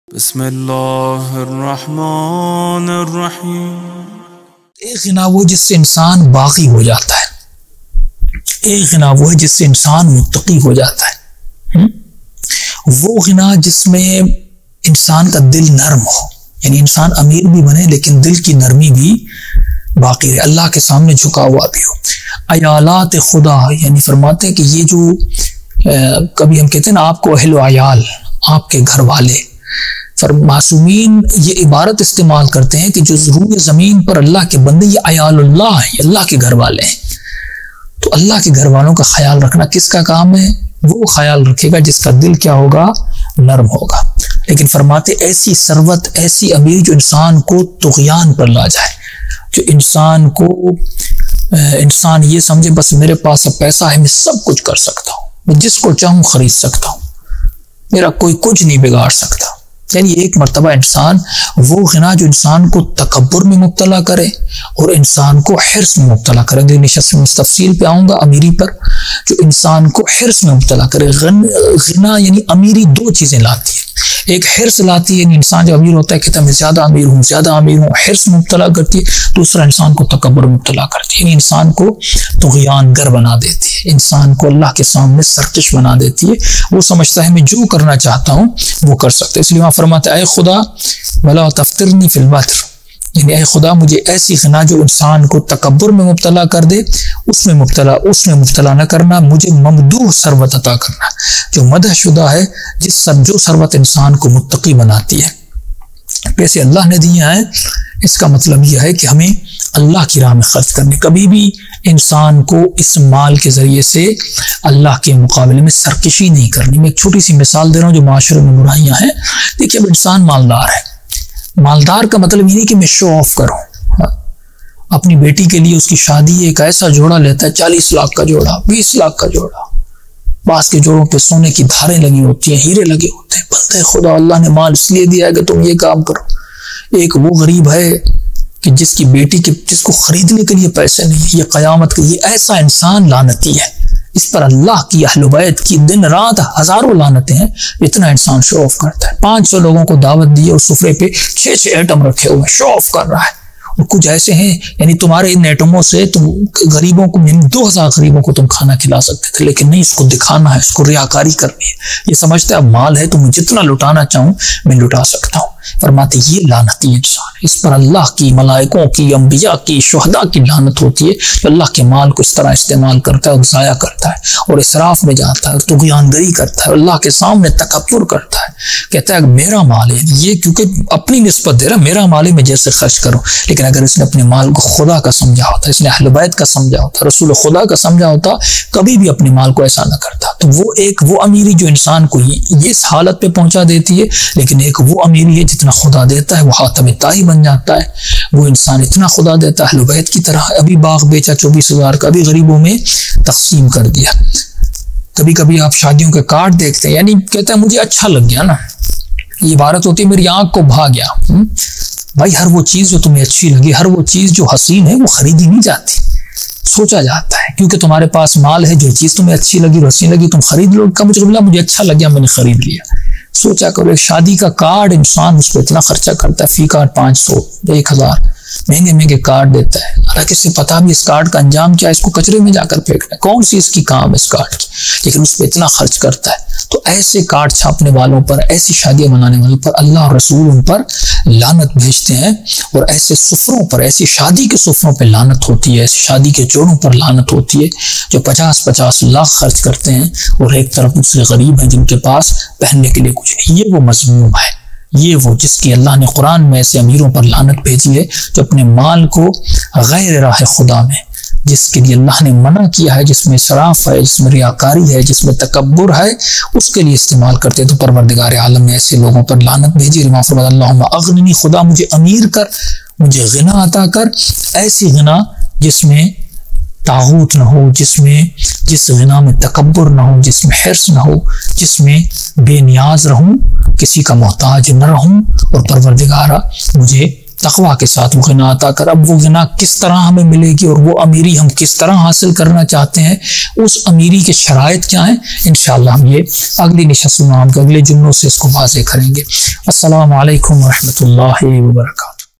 Aysa Ameer Jiss Par Lanat Barasti Rehti Hai | Maulana Syed Arif Hussain Kazmi
Copyright strictly prohibited ⓒ
Please send us back your any of uploaded includes:
Unlisted/private or  published videos For The Necessary Review on: